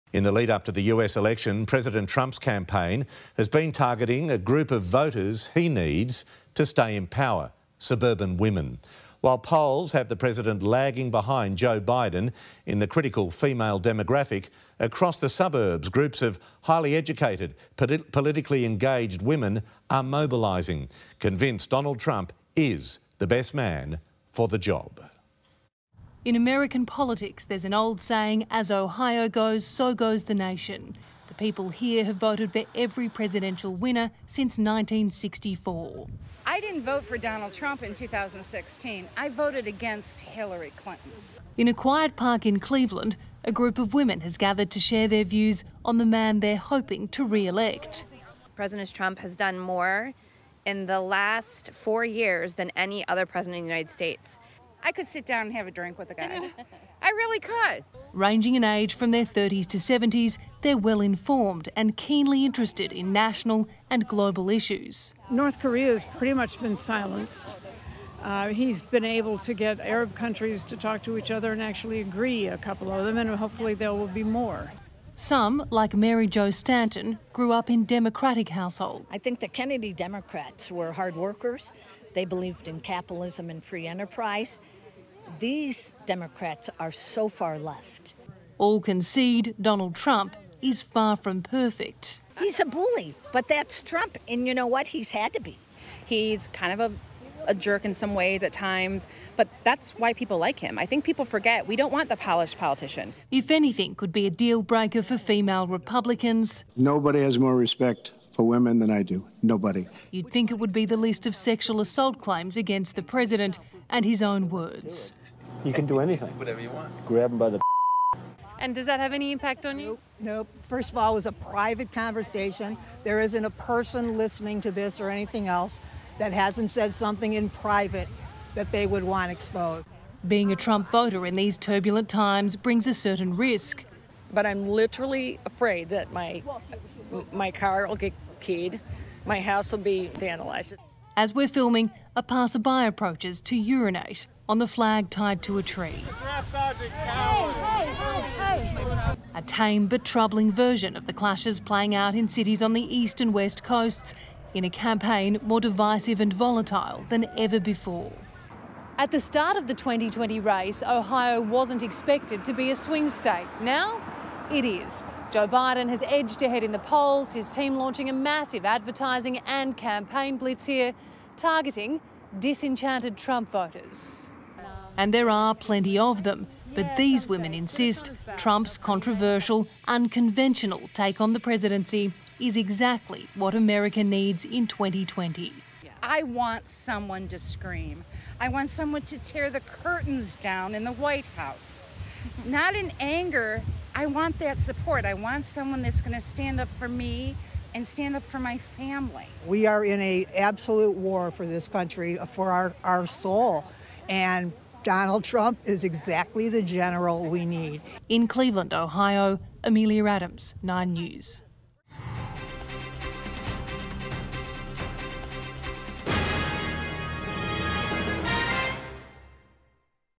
US female demographic vow to vote for Trump | 9 News Australia
The critical female demographic across the US vow to vote for President Donald Trump, convinced he is the best man for the job despite falling behind in the polls.